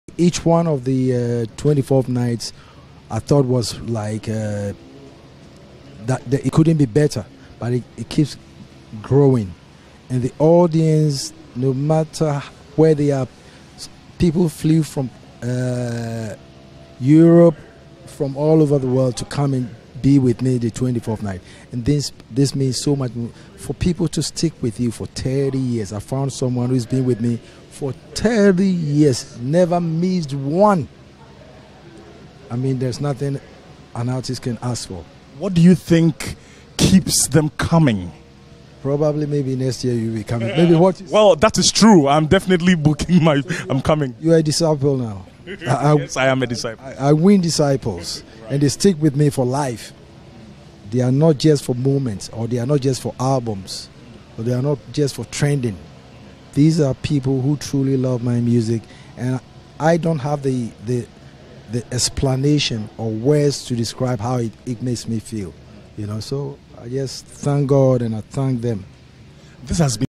I'm a disciple maker - Kojo Antwi
“I win disciples and they stick with me for life. They are not just for moments, albums, nor trends. These are people who truly love my music. I don’t have the words to describe how it makes me feel. I just thank God and I thank them,” Kojo Antwi said this at the 30th edition of the Lovers Celebration Concert.